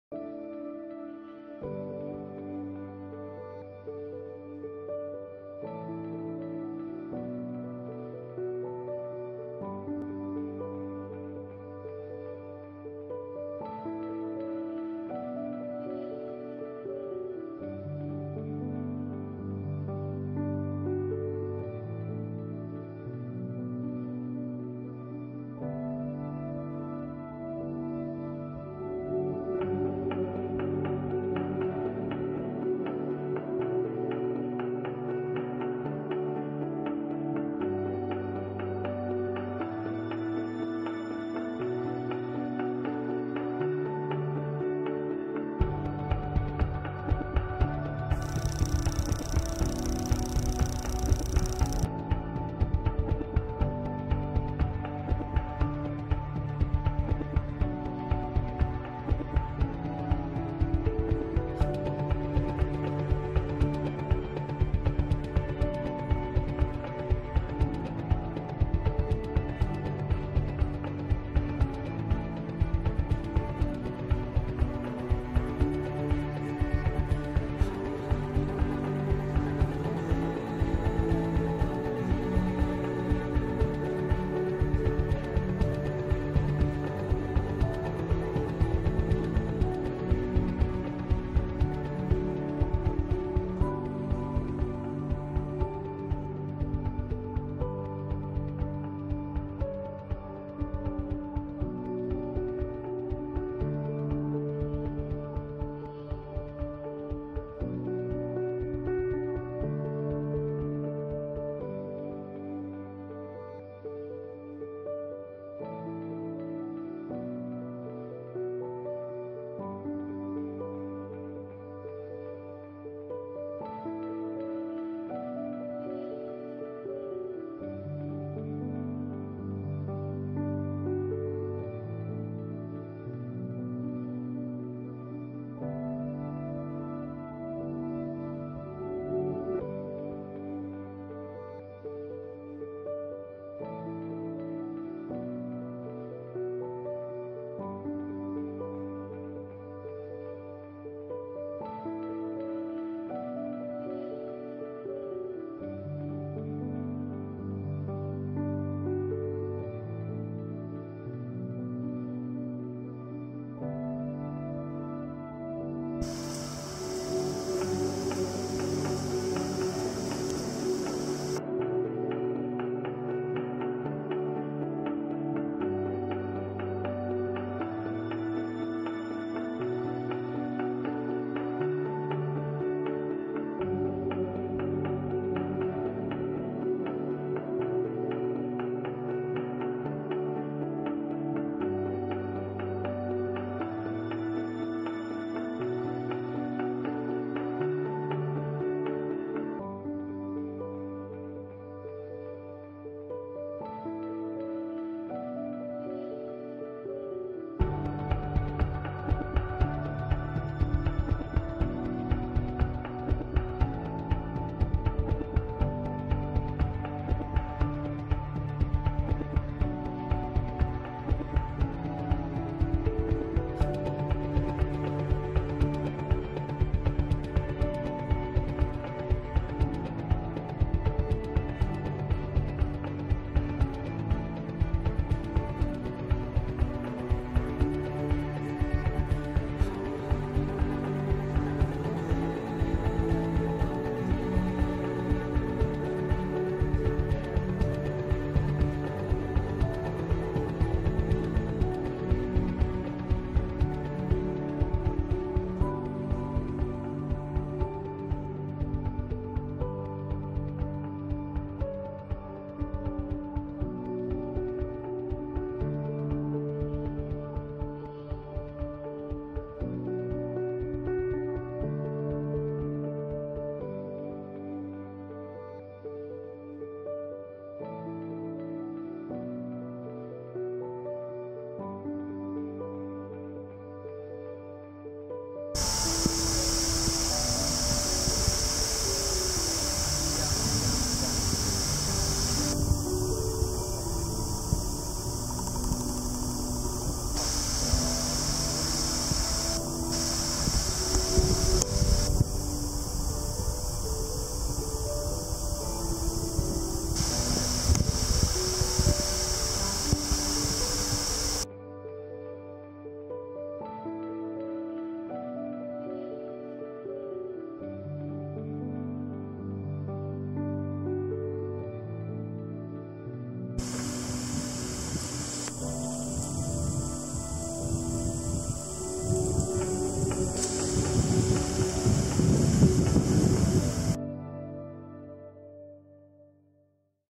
Taiwan, Jiaming Lake to Walami (嘉明湖到瓦拉米)
A 6 day hike from Xiangyang National Forest Rec. Area to Walami via Jiaming Lake, Xinkang Mt, and the Eastern section of the Japanese Colonial Period Batongguan Historical Trail.